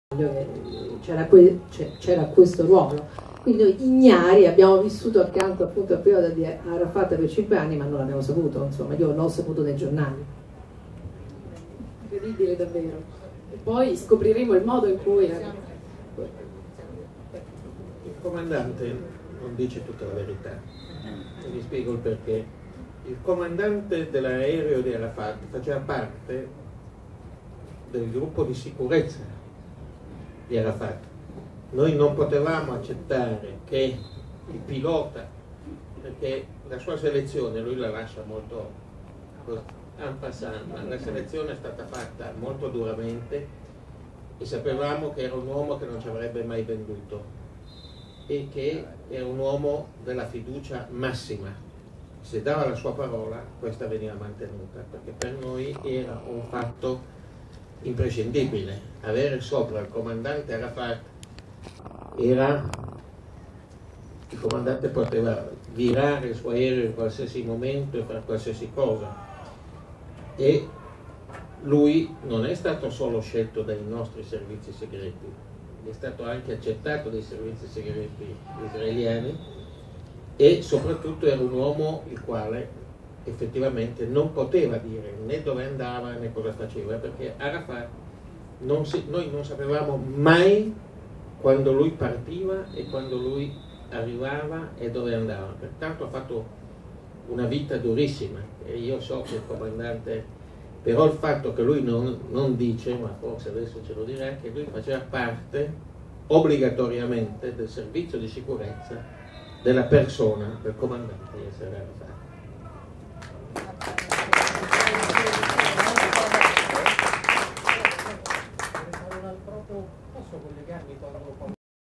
Yasser Arafat - Il suo Pilota Segreto ed il suo Banchiere
La figura di Yasser Arafat nel ricordo del suo banchiere, D'Angieri ambasciatore del Belize e del suo pilota segreto italiano, Marcello Quaglia, sui cui la figlia Lucilla ha scritto un libro.
Aqueistalo qui